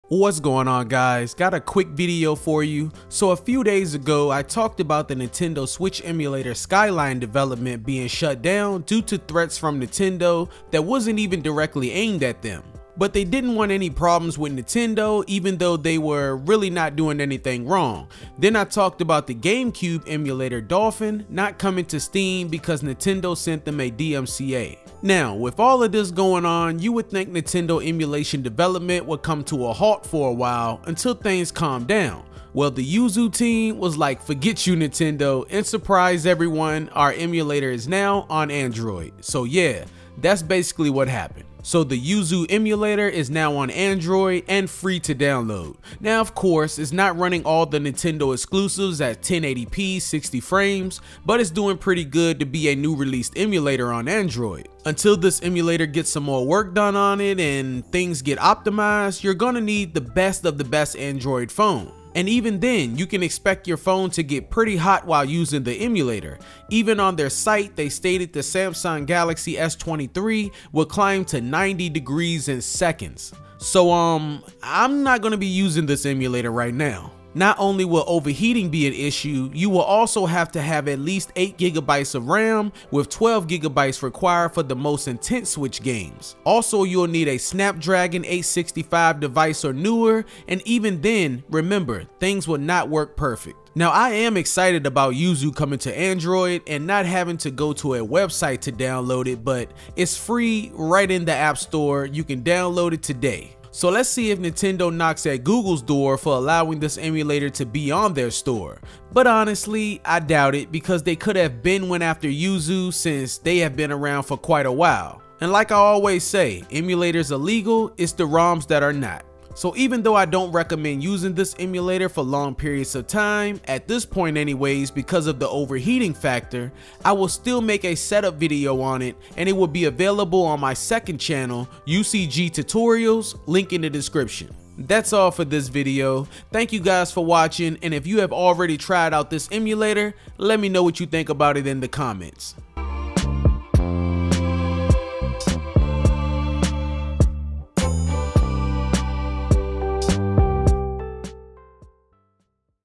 Major Switch Emulator Is Now on Android
You can download the Yuzu emulator on the Google play store